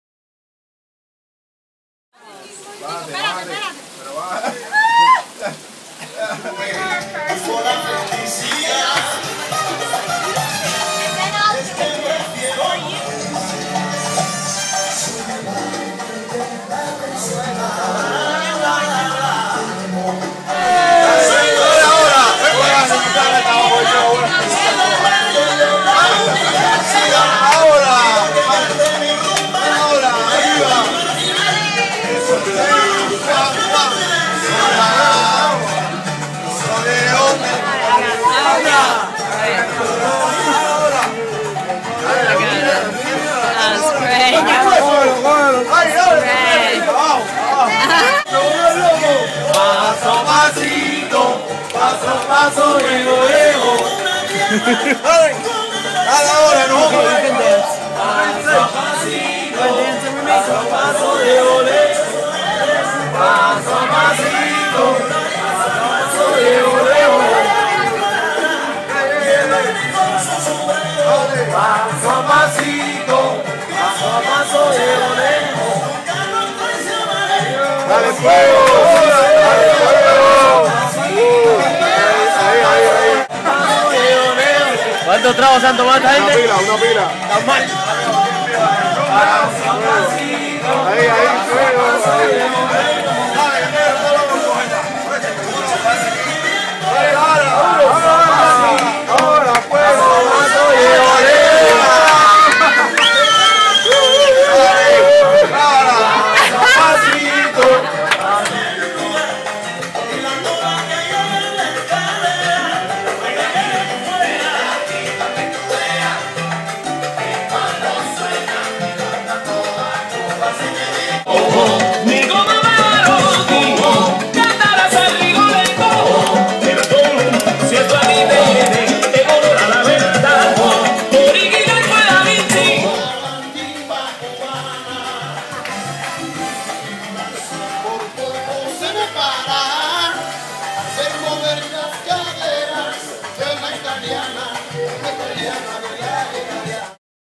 Honeymoon in Cuba
Phil and I were at an outdoor cafe in the afternoon and it started pouring rain. We were rushed into the small shelter of the kitchen with the cafe staff.